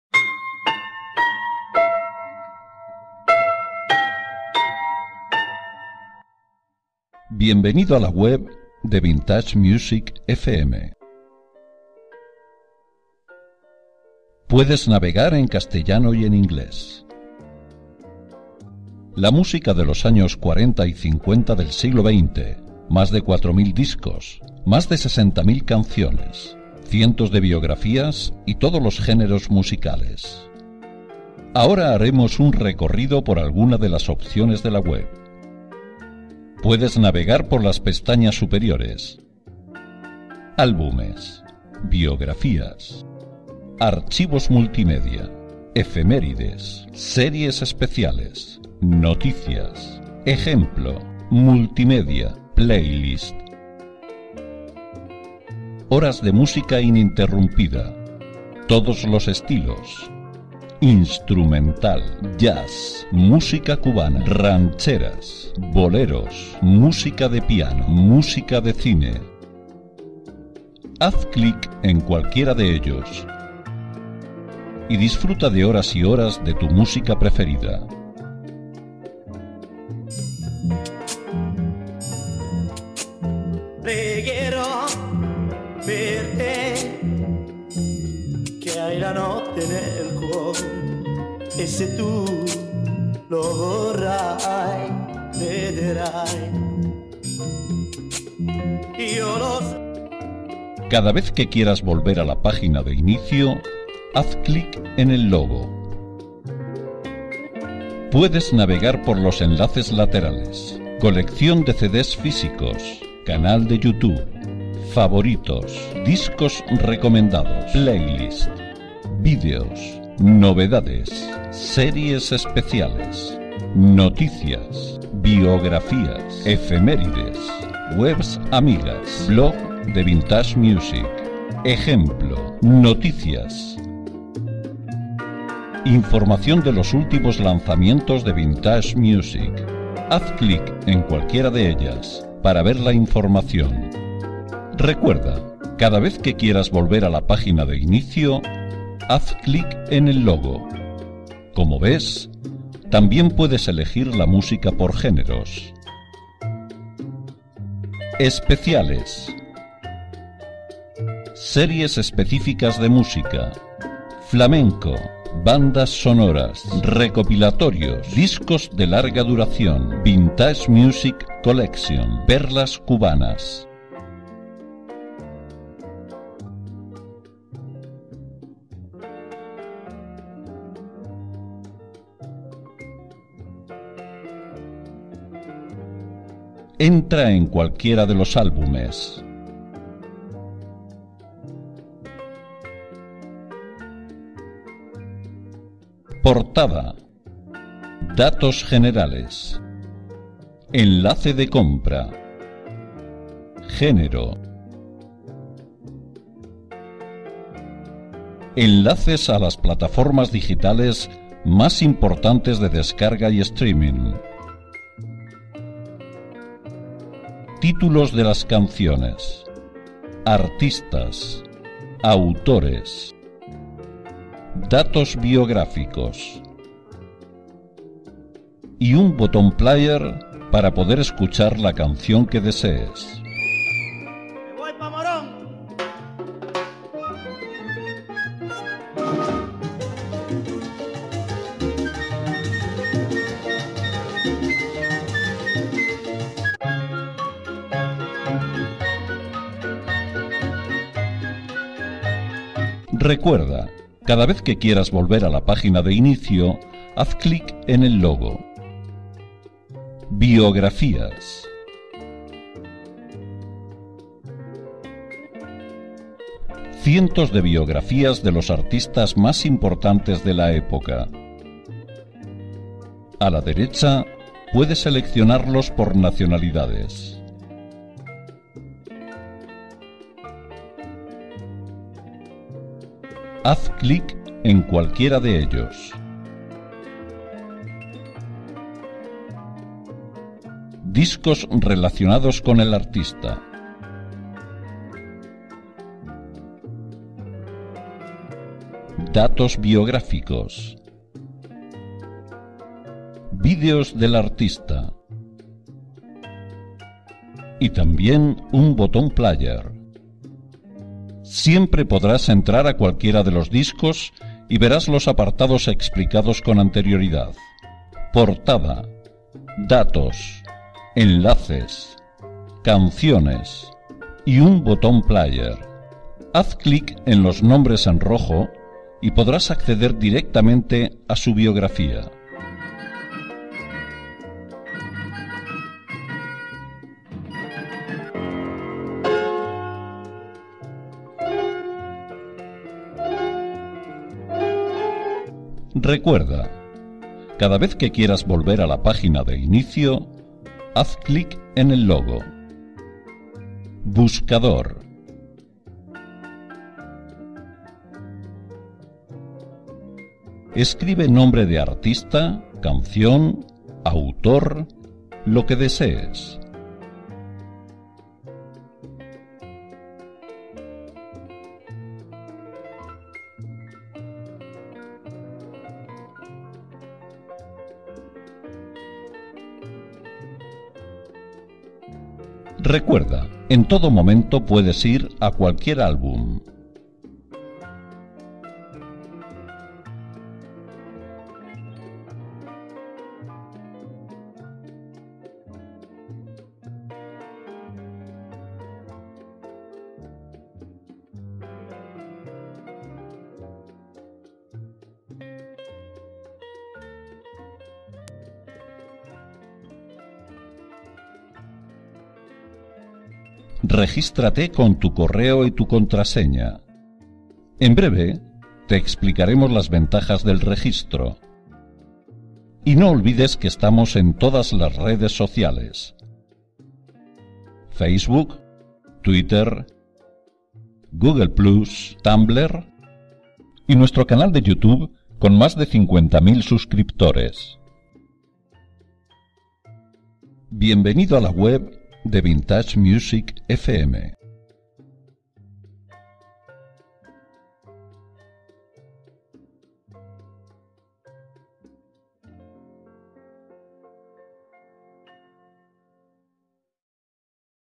Tutorial
¿Quieres exprimir al 100% nuestra web y escuchar desde allí horas y horas de música gratis en Spotify?